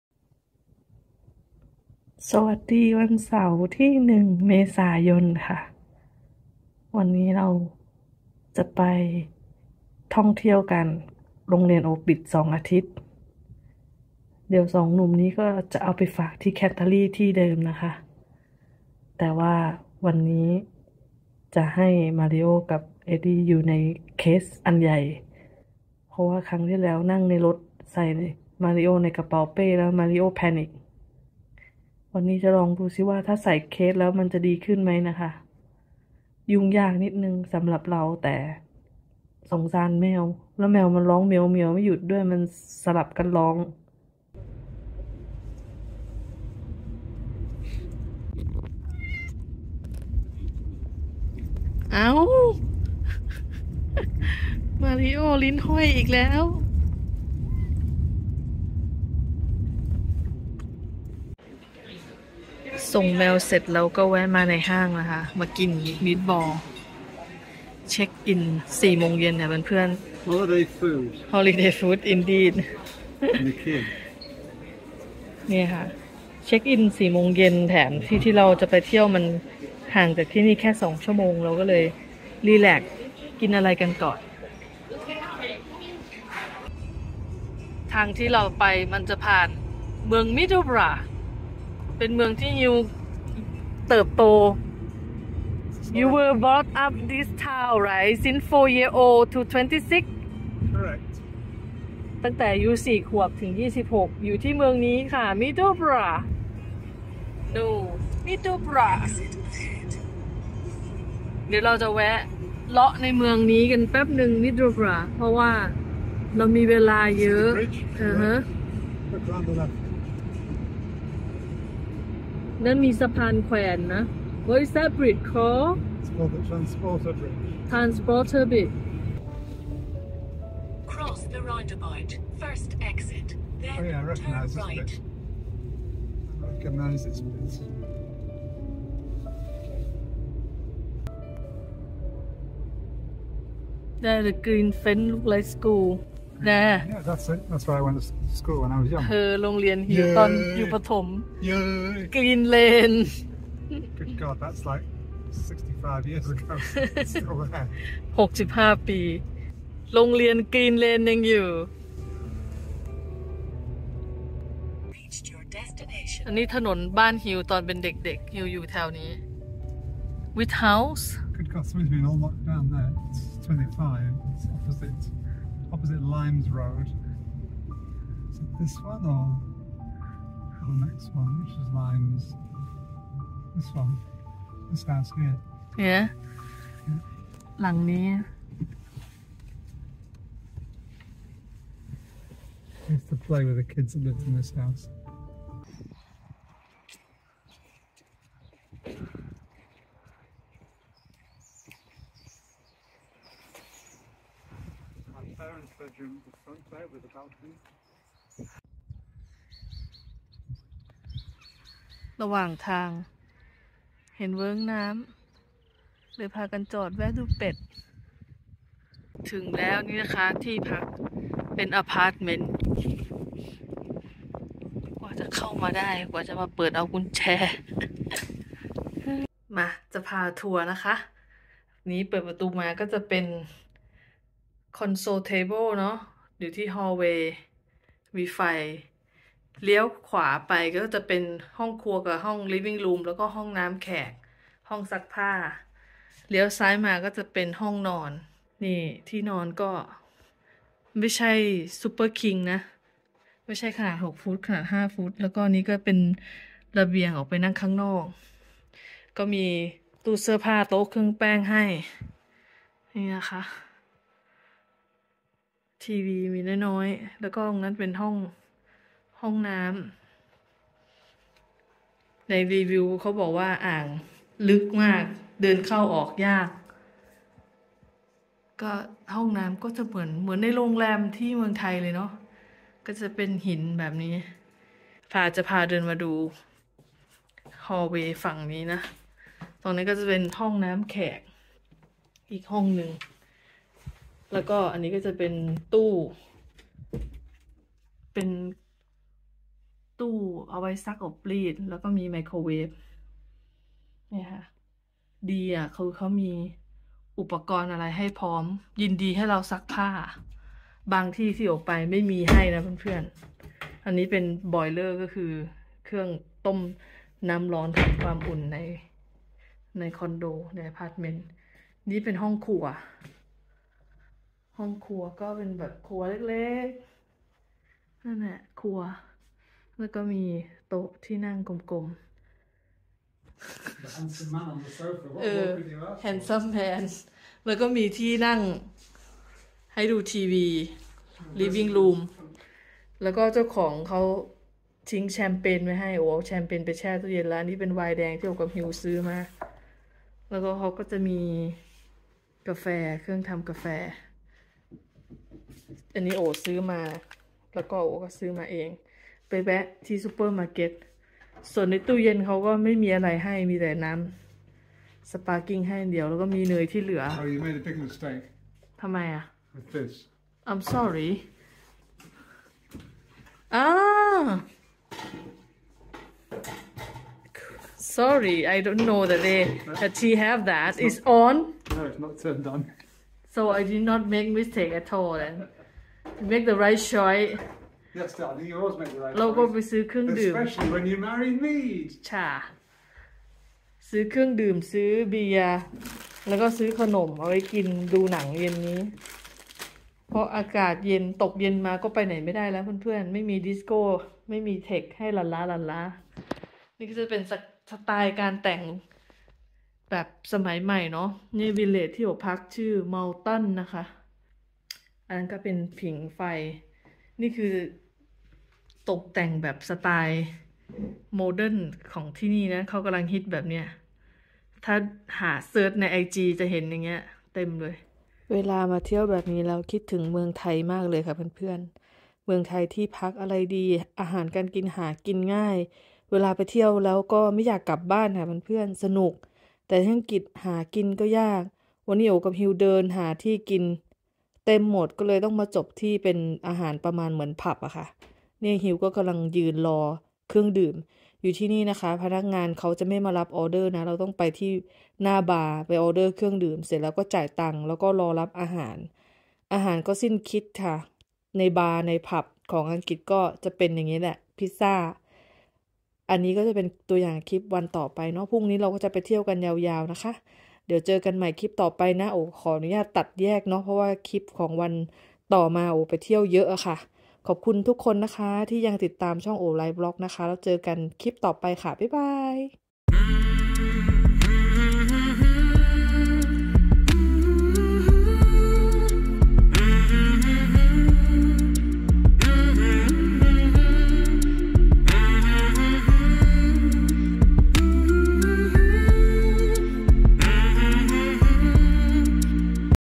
1/4/23 ล้อหมุนเริ่มต้นฮอลิเดย์ที่ Yorkshire, แวะดูบ้านเก่าที่สามีอยู่ตั้งแต่เด็กจนโต
❤️ขอบคุณ​ทุกคนนะคะที่แวะมารับชมคลิปนี้​  ฝากกด Like , กด Shares และกดติดตามด้วยเน้อ  ขอบคุณค่ะ  ❤️️🙏🌻🌺🌷🌸😊